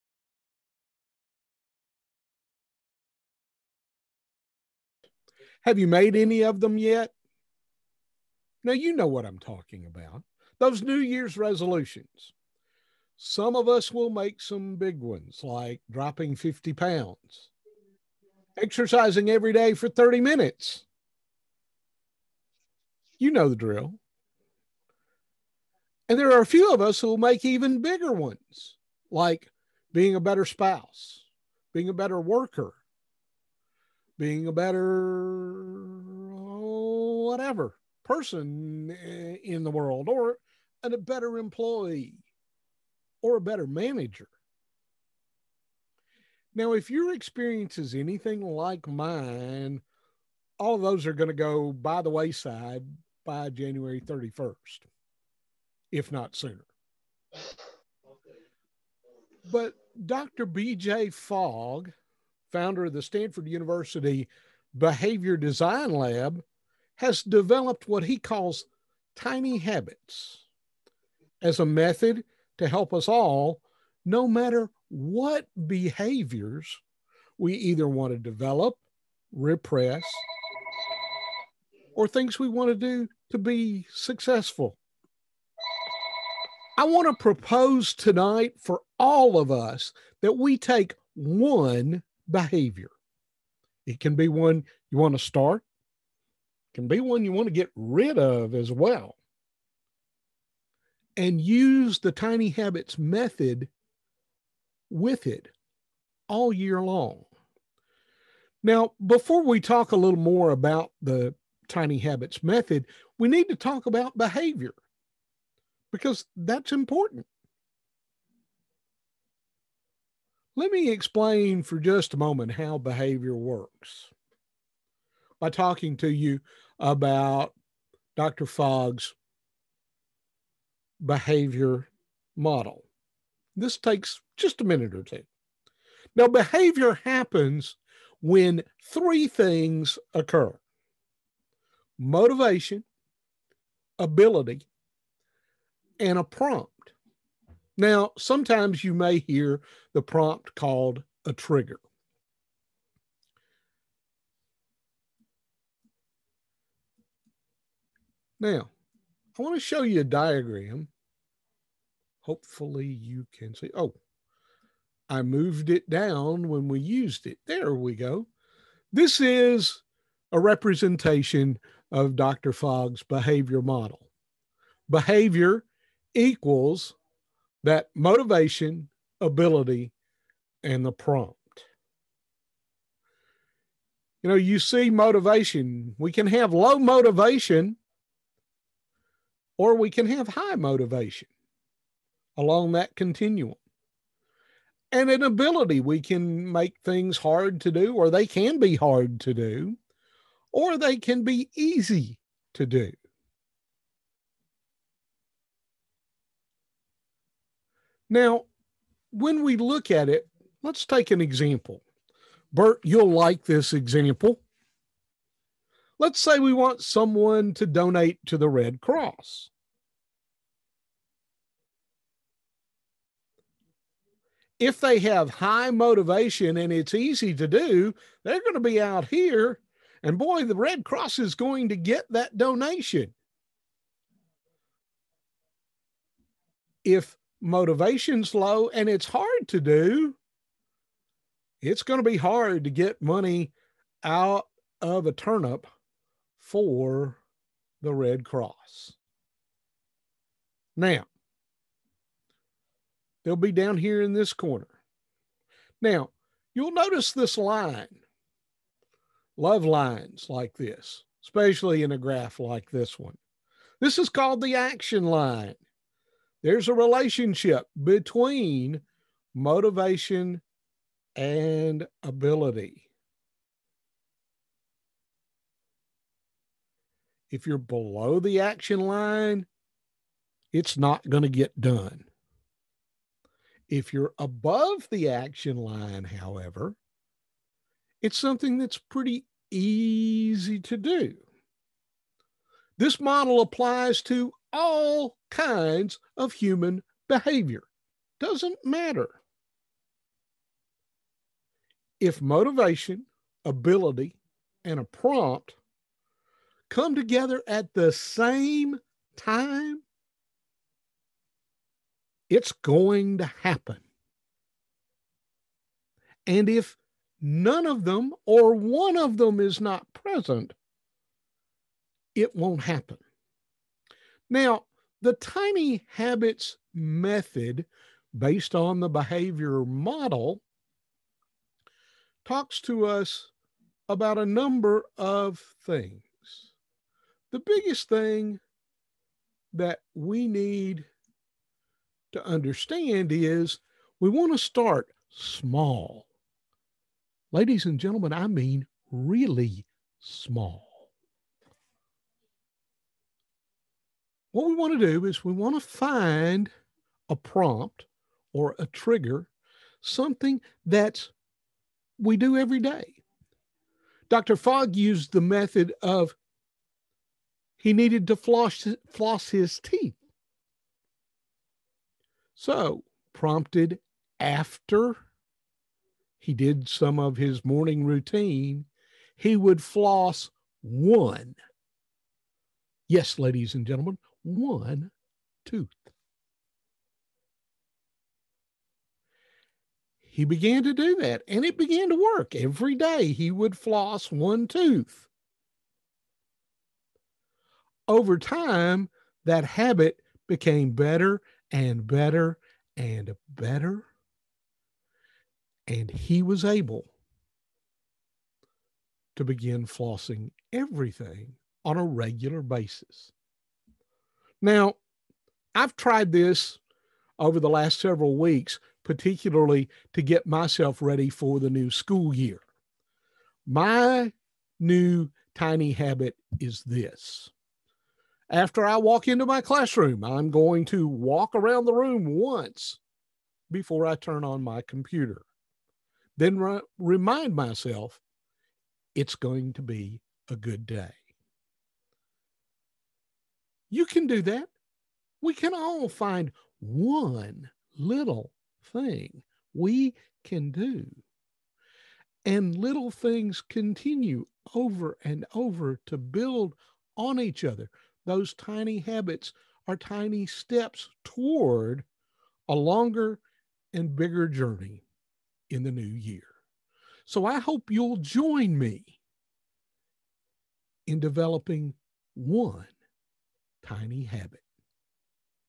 Tom Beesely Speech 29 Dec 2020
Goals speech by Tom Beesley 29 Dec 2020 Springfield Igniters Meeting